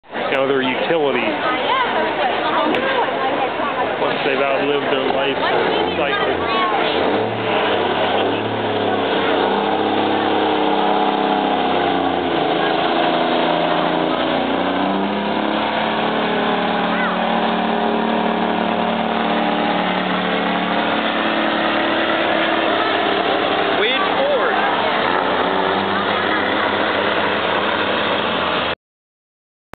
red white kenworth truck pull
red white kenworth truck pull at portage county randolph fair 8-24-10